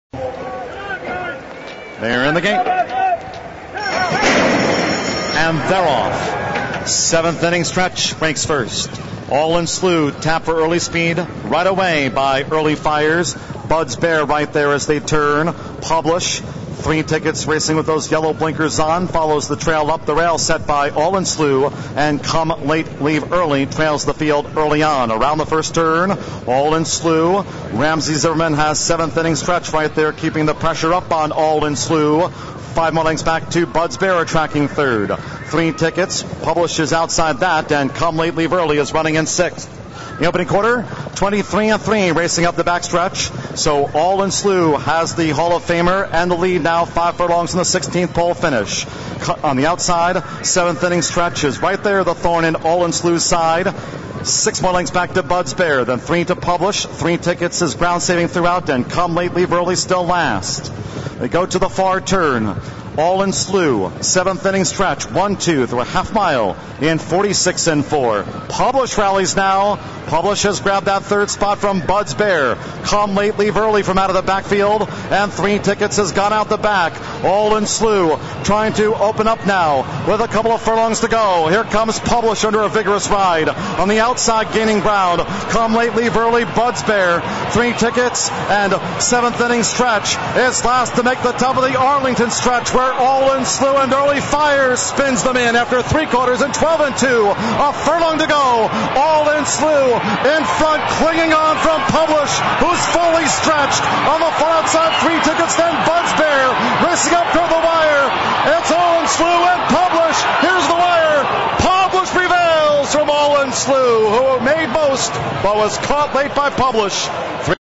ARLINGTON PARK, 2008-07-20, Race 8
Entries: Thoroughbred Race, ARLINGTON PARK, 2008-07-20, Race 8:
      (Pgm-Runner-Jockey-Finish)
       1-Three Tickets (IL), Alvarado, J, finished 3;
       3-Comelateleavearly (IL), Sterling, Jr., L, finished 5;
       4-All in Slew (IL), Fires, E, finished 2;
       5-Sevnthiningstretch (IL), Zimmerman, R, finished 6;
       7-Publish (IL), Graham, J, finished 1;
      11-Bud's Bear (IL), Riggs, T, finished 4;